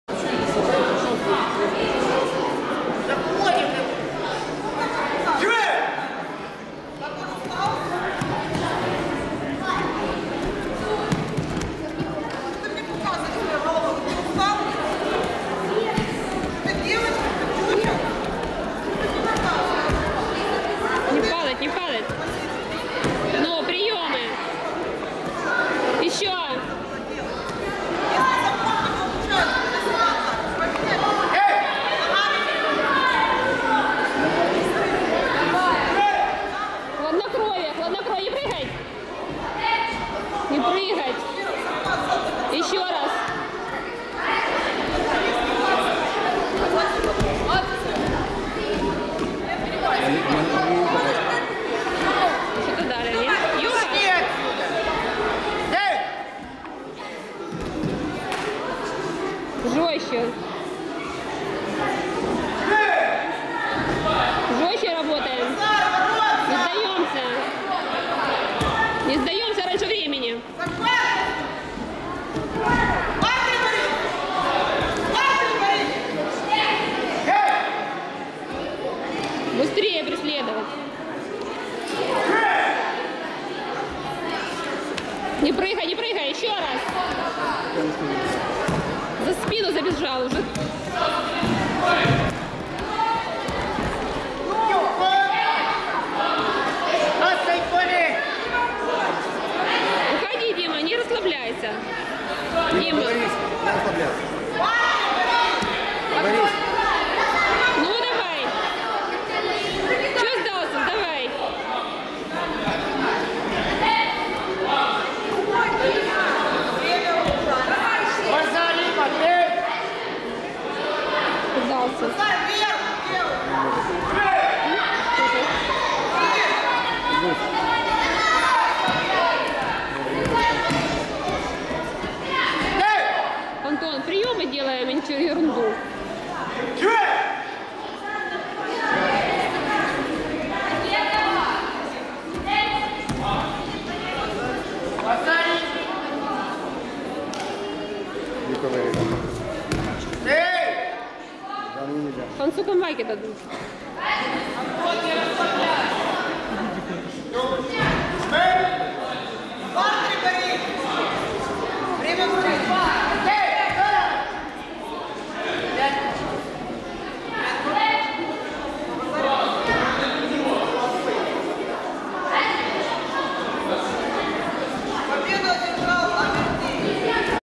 Обертий W- Шумак L -32 кгОткрытое первенство ЦСДЮШОР по дзюдо среди юношей 2002-2003 г.р
Обертий W- Шумак L Открытое первенство ЦСДЮШОР по дзюдо среди юношей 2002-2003 г.р
19 октября 2013